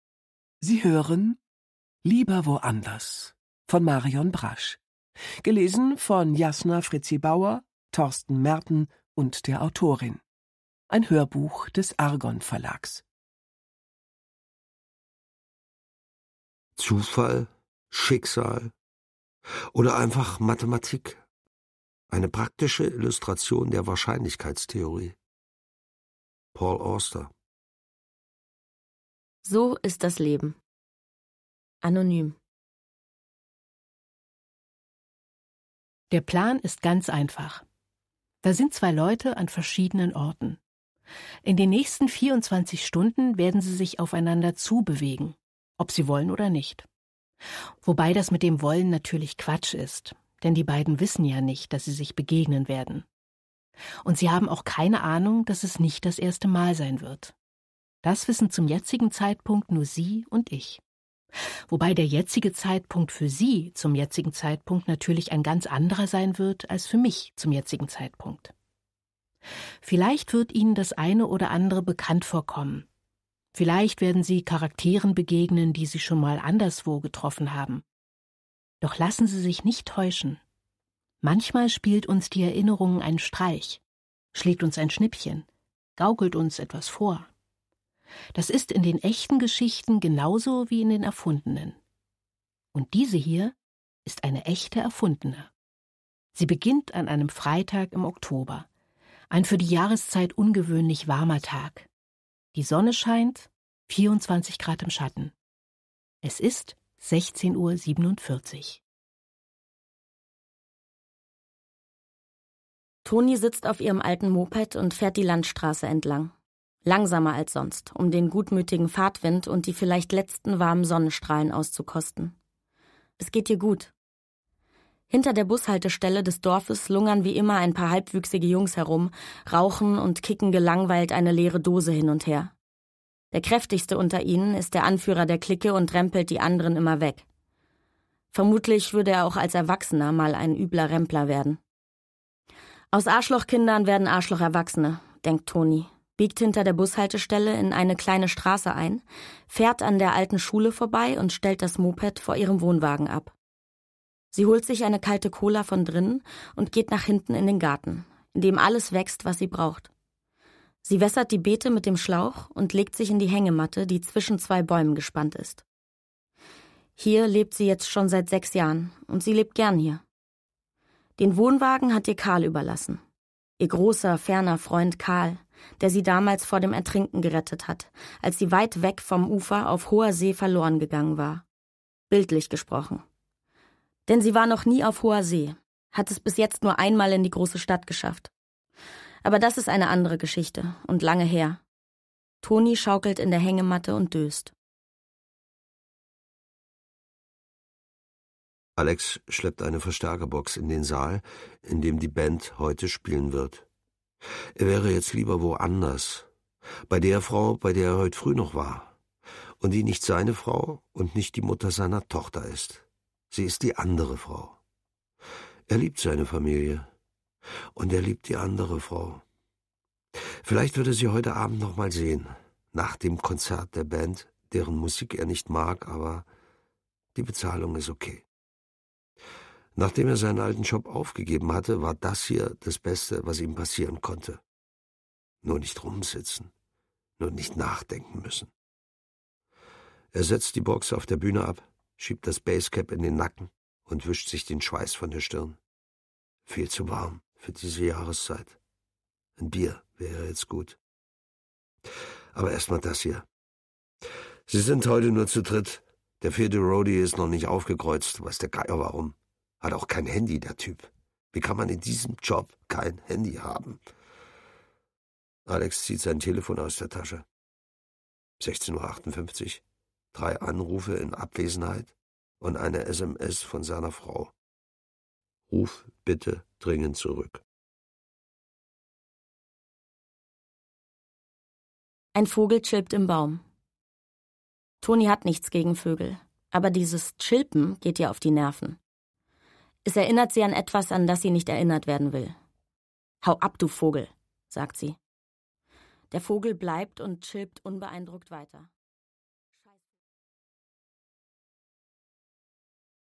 Marion Brasch - Lieber woanders
Inhaltsangabe:
Toni und Alex kennen sich nicht und sind doch auf verhängnisvolle Weise miteinander verbunden. Toni leidet unter dem Verlust ihres kleinen Bruders, für dessen Tod sie sich verantwortlich fühlt. Alex führt ein Doppelleben und trägt an einer Schuld, über die er nie gesprochen hat. 24 Stunden bewegen sich die beiden aufeinander zu, bis sich schließlich ihre Wege trotz skurriler Begegnungen und komischer Zwischenfälle kreuzen. Marion Brasch erzählt diese Geschichte vom Leben und Überleben in einem klaren, warmen Ton und mit großem Gespür für die Augenblicke im Leben, die über Glück oder Unglück entscheiden.